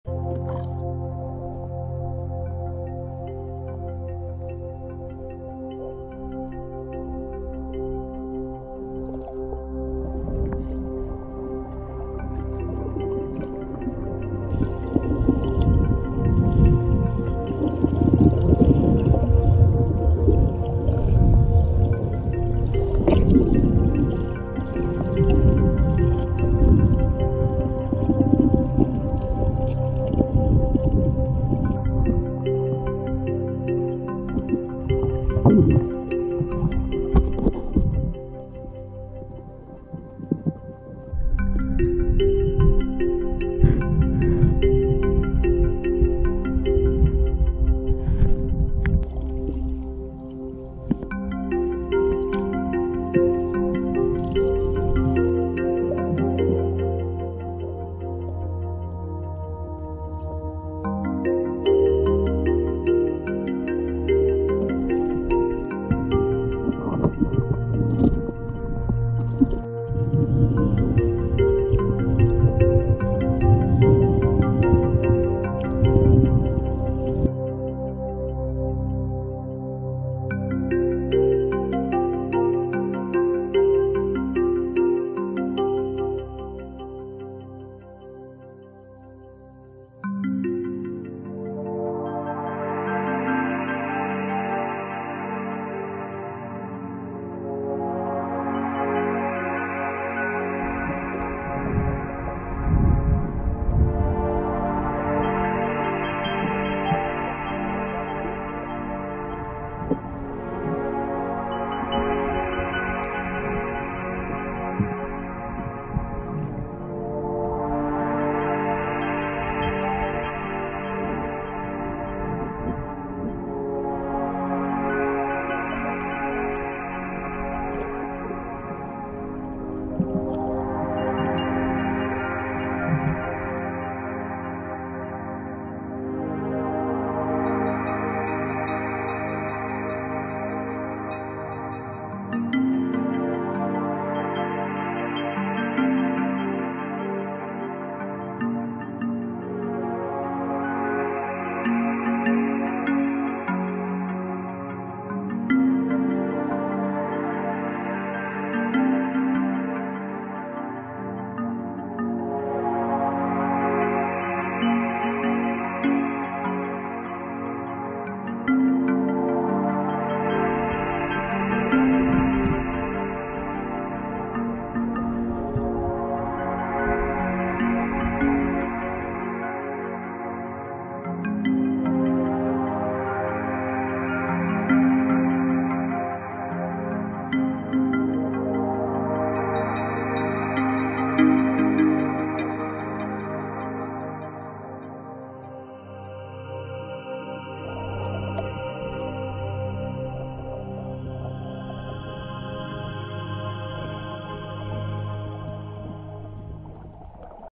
Eit dykk i havet - Drønsstraumen
MUSIC DOES NOT BELONG TO ME, LINK BELOW, THANK YOU!
Eit flott dykk i Drønsstraumen i dag 30.Juli.
Free diving in Norway will give you a very rich experience. Today we went diving in "Drønsstraumen".